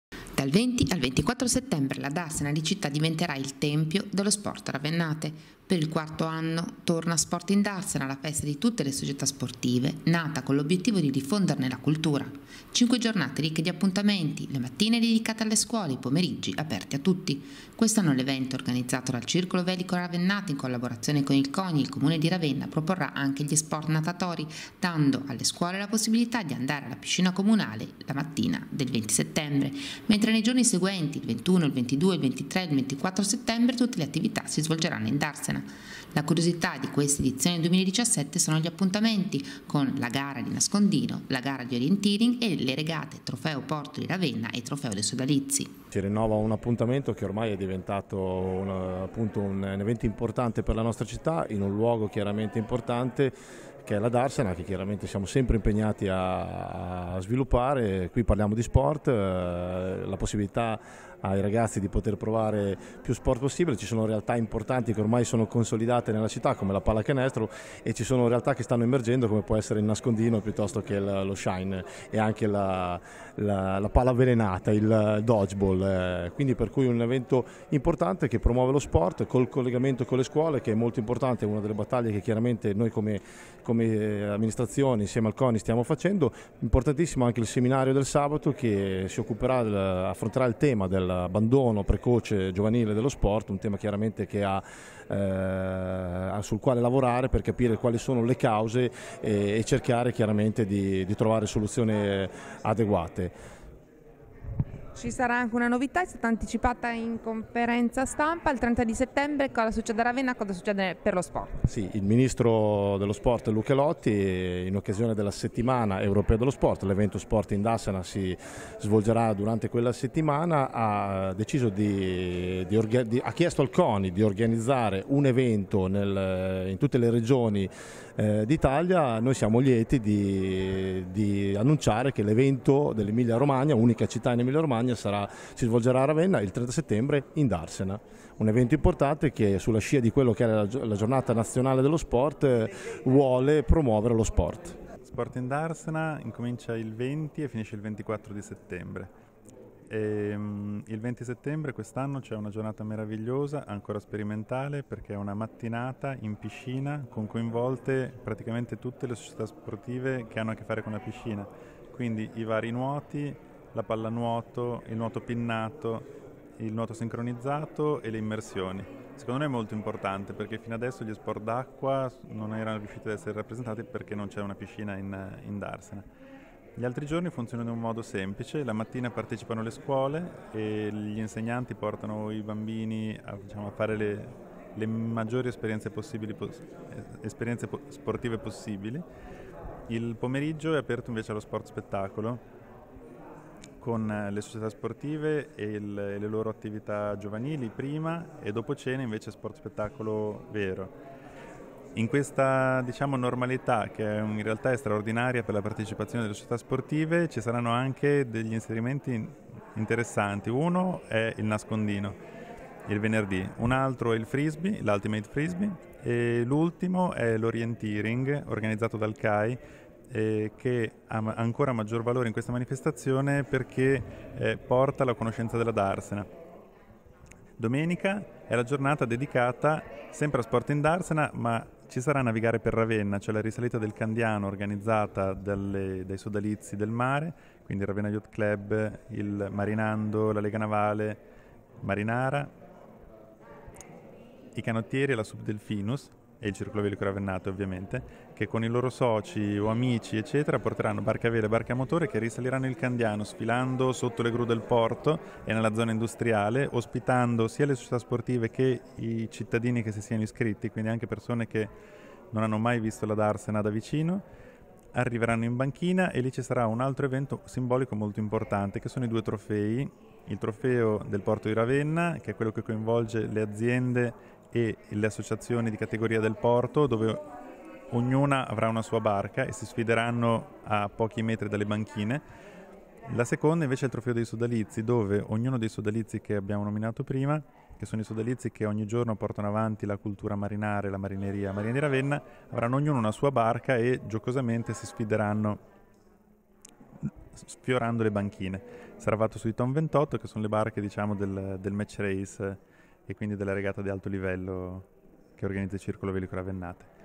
Dal 20 al 24 settembre lo Sport invade la Darsena
13/09/2017 - Dal 20 al 24 settembre la Darsena di città diventerà "tempio" dello sport ravennate. Per il quarto anno torna Sport in Darsena, la festa di tutte le società sportive, nata con l'obbiettivo di diffonderne la cultura. Cinque giornate ricche di appuntamenti, le mattine dedicati alle scuole e i pomeriggi aperti a tutti. Quest'anno l'evento, organizzato dal Circolo Velico Ravennate, in collaborazione con il Coni e il Comune di Ravenna, proporrà anche gli spot natatori, dando alle scuole la possibilità di andare alla piscina comunale, il 20 mattina, mentre nei giorni seguenti 21, 22, 23, e 24 settembre tutte le attività si svolgeranno in Darsena. La curiosità di questa edizione 2017 sono gli appuntamenti con la gara di nascondino, la gara di orienteering e la Regata Trofeo Porto di Ravenna e il Trofeo dei sodalizi.